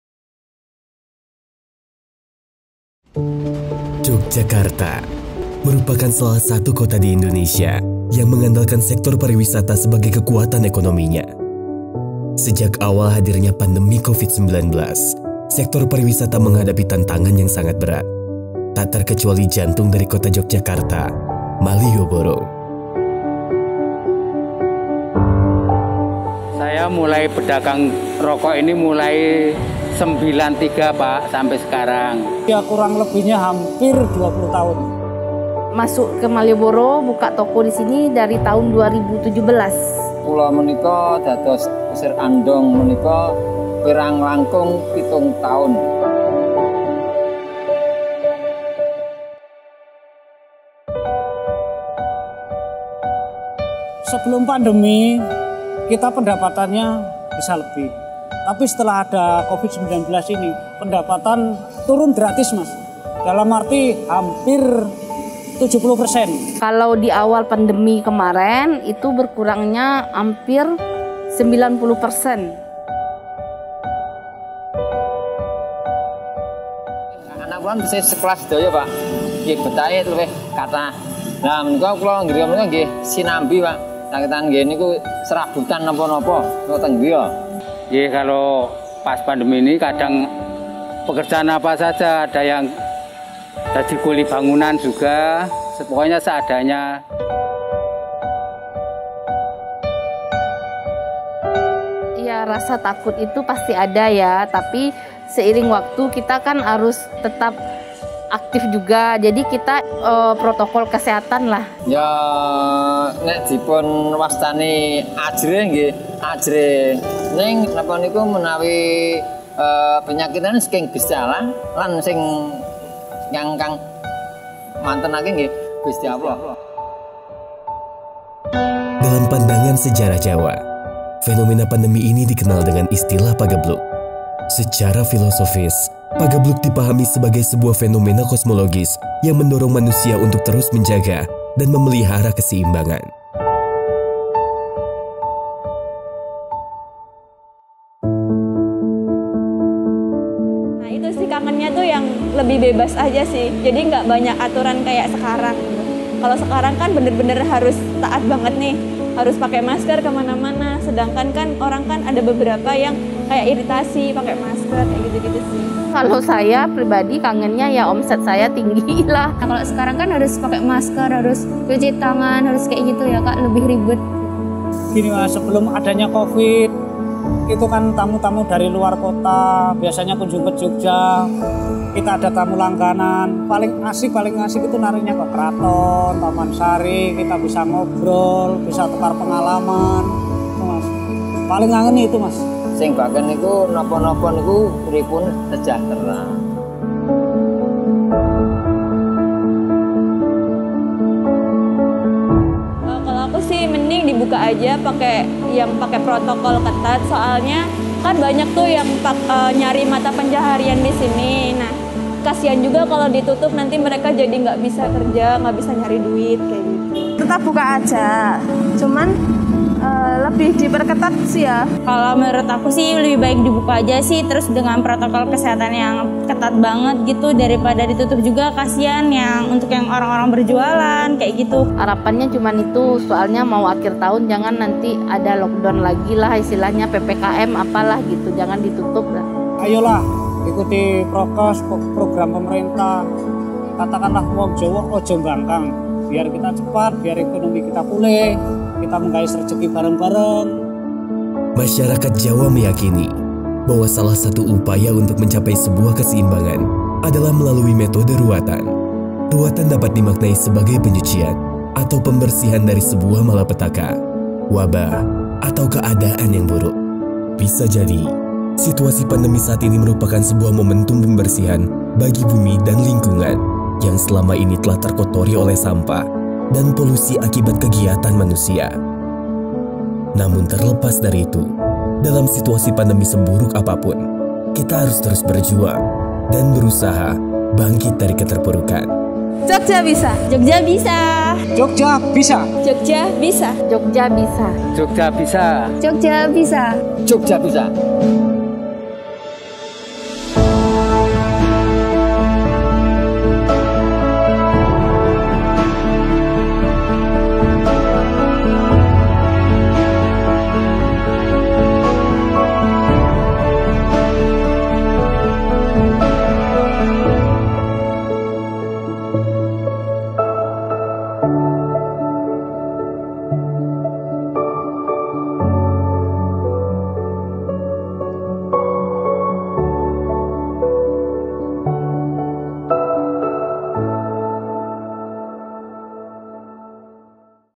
PAGEBLUK | Short Documentary
Jogjakarta, merupakan salah satu Kota di Indonesia yang mengandalkan sector pariwisata sebagai kekuatan ekonominya. Sejak awal hadirnya pandemi Covid 19 sektor pariwisata menghadapi tantangan yang sangat berat, tak terkecuali jantung dari Kota Jogjakarta, Malioboro. Hal ini menyebabkan berbagai usaha yang mengandalkan pendapatan dari kunjungan wisatawan otomatis mengalami penurunan drastis. Pandemi Covid 19 termasuk dalam bencana non alam yang memberikan dampak signifikan bagi seluruh sektor kehidupan. Mulai dari sektor kesehatan, ekonomi, pendidikan, keagamaan, dan sektor lain terkena imbasnya. Masyarakat Jawa pada khususnya, mengenal fenomena pandemi ini dengan istilah “pagebluk”. Dalam pandangan sejarah dan peradaban Jawa, pagebluk dipahami sebagai sebuah fenomena kosmologis yang mendorong manusia untuk terus memelihara keseimbangan. Keseimbangan adalah keselarasan dalam diri pribadi, antara manusia dengan manusia yang lain, manusia dengan lingkungan, serta manusia dengan Tuhan. Salah satu upaya masyarakat Jawa untuk mencapai sebuah keseimbangan adalah melalui metode “Ruwatan”. Ruwatan secara luas dapat dimaknai sebagai sebuah penghancuran, pembebasan, penyucian atau pembersihan dari sebuah malapetaka, wabah atau keadaan yang buruk. Pada dasarnya setiap entitas di muka bumi perlu mengalami sebuah fase “pembersihan”, termasuk bumi dan lingkungan itu sendiri. Pandemi memang berdampak buruk, tetapi jika kita mau menelisik lebih dalam, bisa jadi situasi pandemi ini merupakan sebuah momentum “pembersihan” bagi bumi dan lingkungan, yang selama ini telah terkotori oleh sampah dan pulusi akibat kegiatan manusia. Seperti pepatah Jawa mengatakan “Ngundhuh Wohing Pakarti”, yang bermakna bahwa apa yang terjadi pada diri kita saat ini tidak lain adalah buah dari perbuatan kita di masa yang lalu, perbuatan baik maupun buruk pasti ada balasannya. Namun terlepas dari apapun itu, selain sebagai momentum refleksi diri, dalam situasi pandemi ini kita juga harus terus berjuang dan berusaha bangkit dari keterpurukan. Kerja sama yang sinergis antara masyarakat, pemerintah dan pelaku industri merupakan kunci utama untuk mempercepat proses pemulihan di segala bidang.